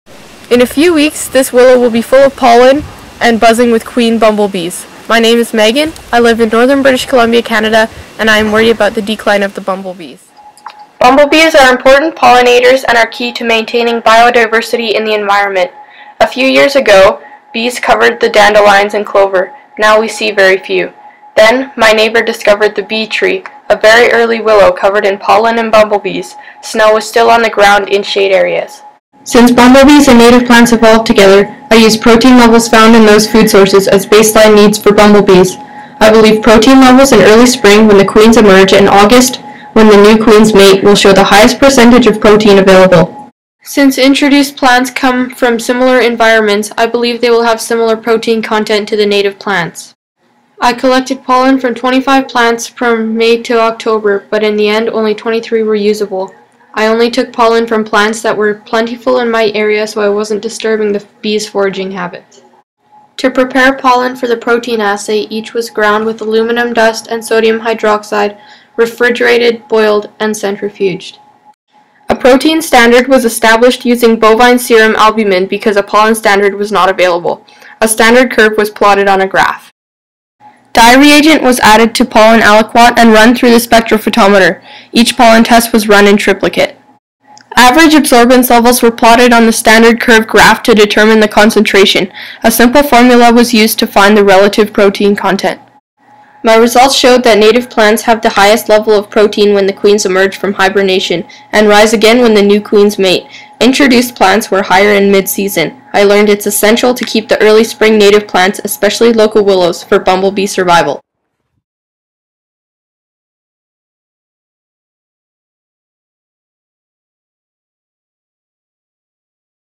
Feeding the Bumblebees.mov
My Science Fair project.  I was trying to see if the protein in the pollen in introduced plants were different than the protein from pollen in native plants in the peace river area.